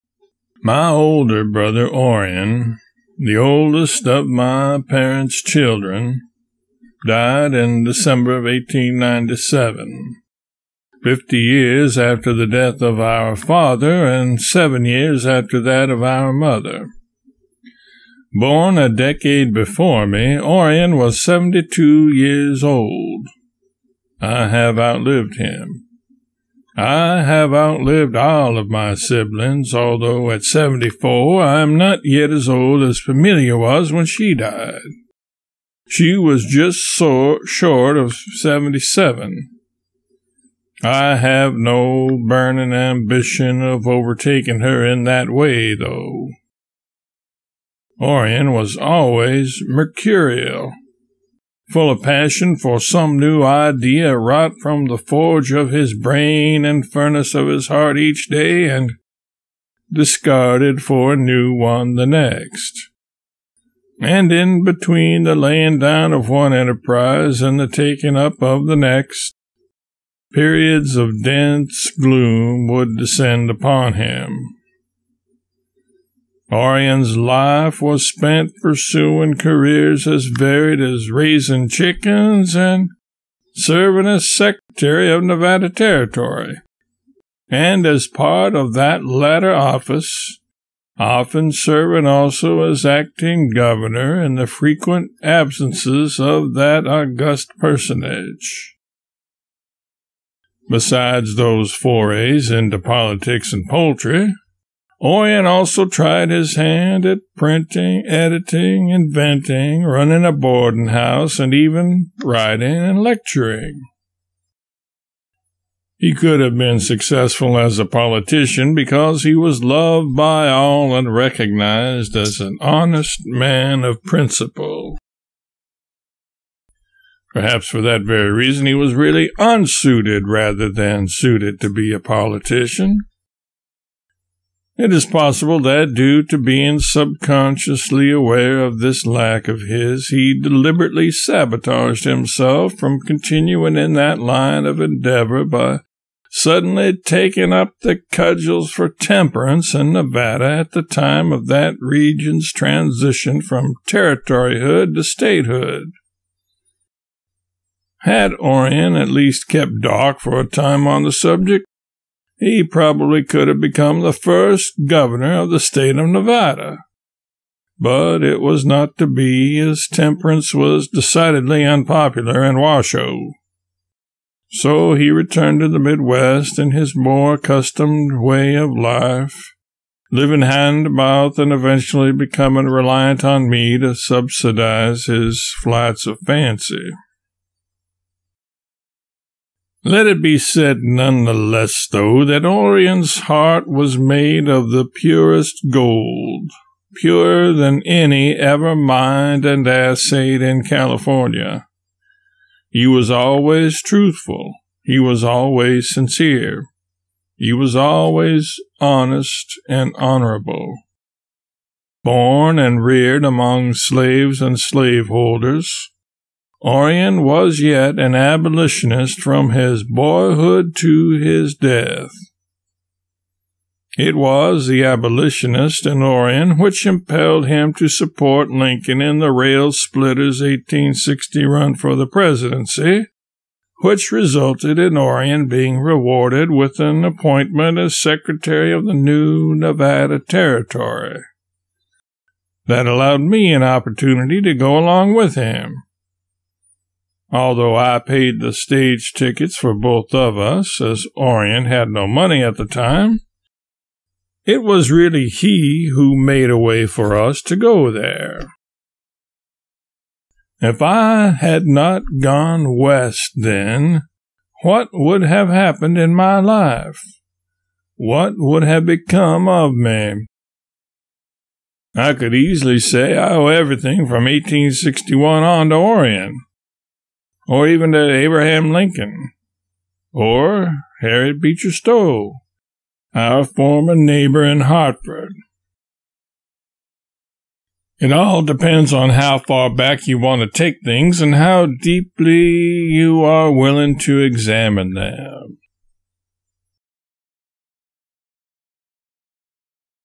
Rebel Twain Chapter 64
Chapter 64 of "Rebel With A Cause: Mark Twain's Hidden Memoirs" - a concise biography of Mark Twain masquerading as a newly-found autobiography